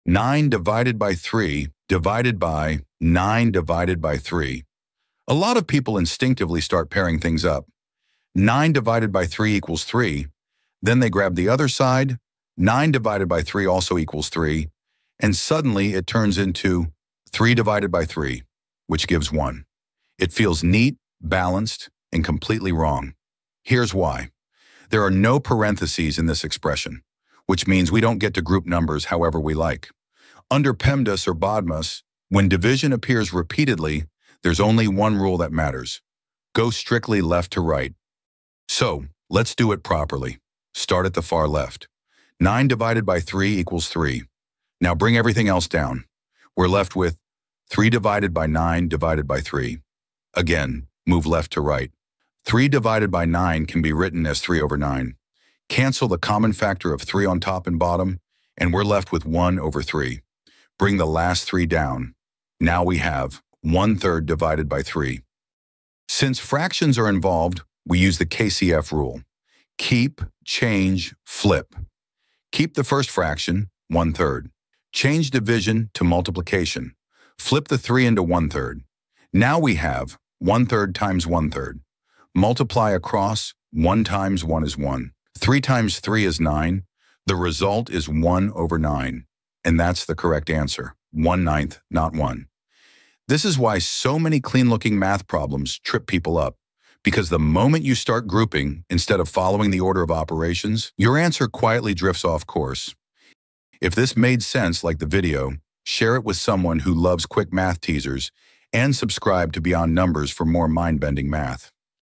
This Division Problem Looks Easy — Until You Try It
At first glance, this division problem looks harmless:

Nine ÷ three ÷ nine ÷ three.

Most people instinctively start pairing numbers and confidently arrive at the wrong answer. In this video, we break down exactly where that mistake happens — and how following PEMDAS or BODMAS the right way leads to the correct solution.

When division appears repeatedly, grouping is not allowed. The only rule that matters is moving strictly from left to right.

Watch till the end to see why clean-looking math problems are often the most deceptive.